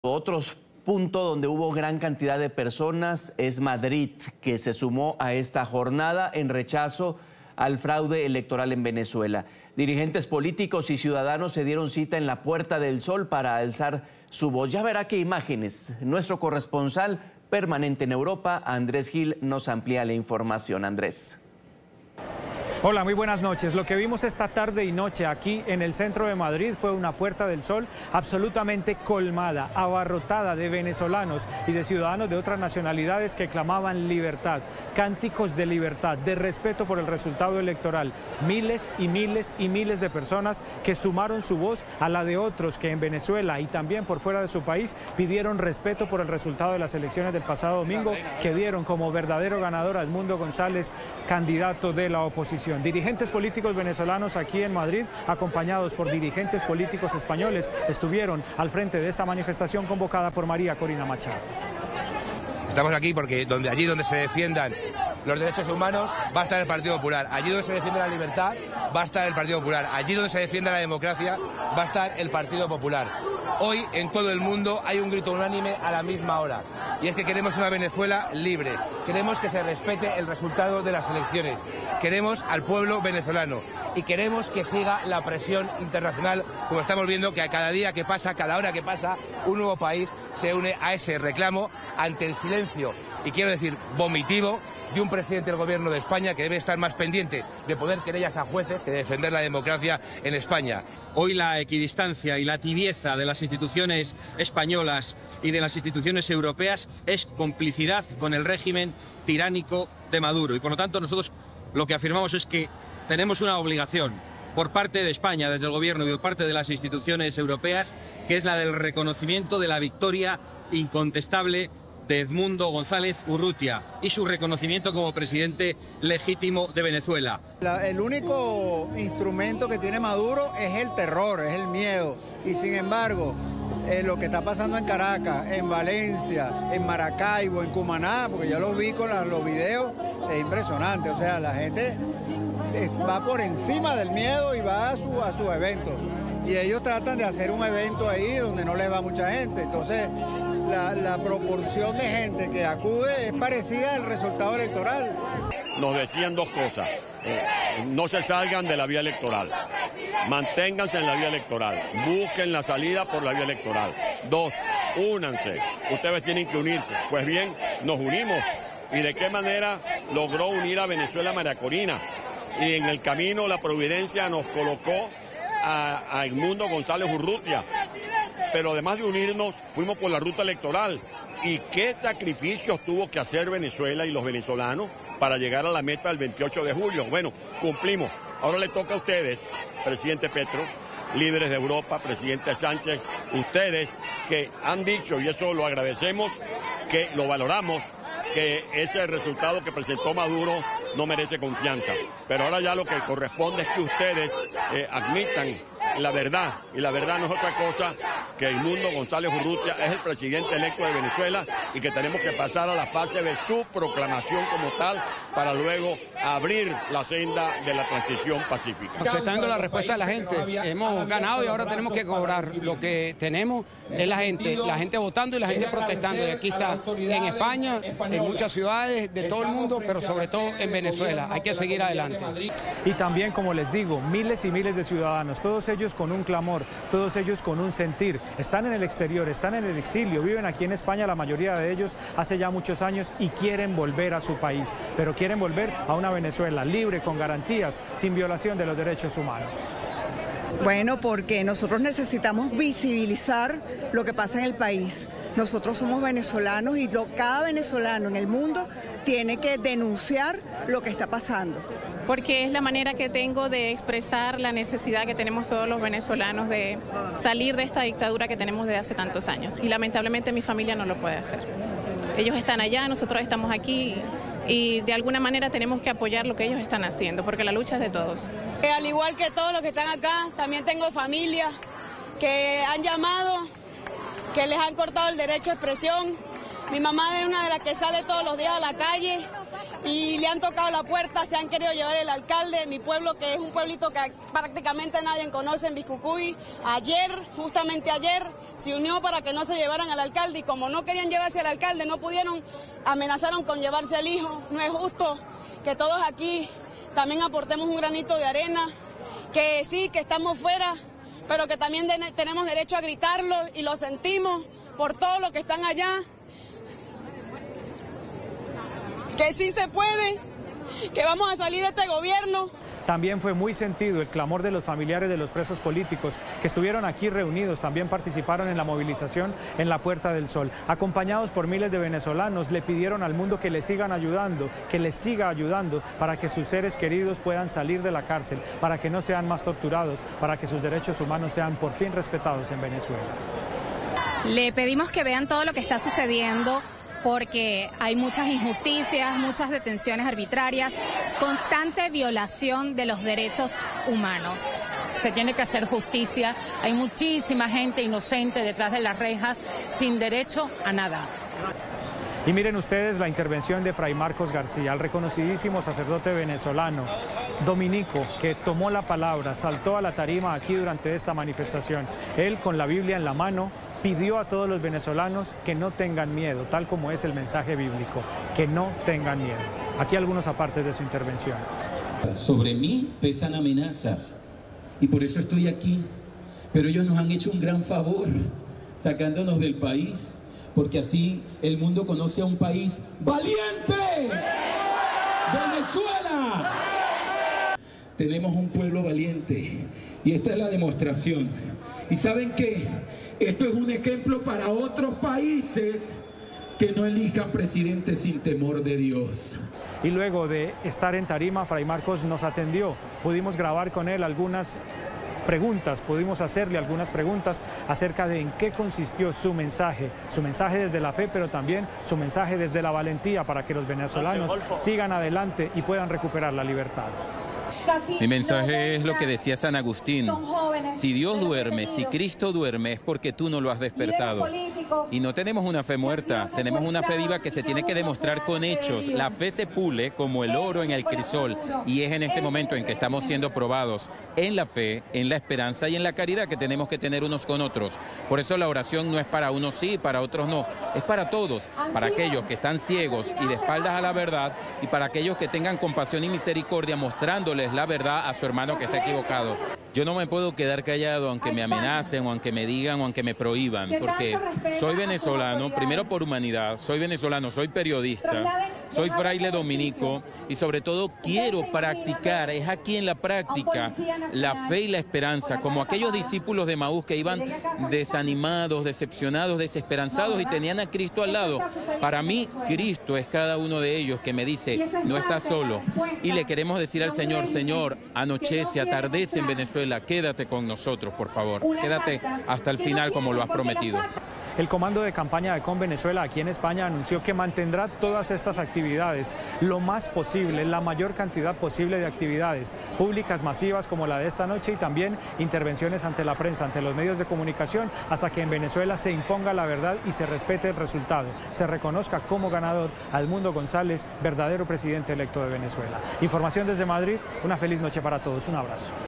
Miles de venezolanos colmaron la Puerta del Sol de Madrid clamando por la libertad de Venezuela
En la Puerta del Sol de Madrid, en España, miles de venezolanos se congregaron para clamar por la libertad de su pueblo y el respeto por la democracia, pues consideran que hubo fraude electoral por parte del régimen.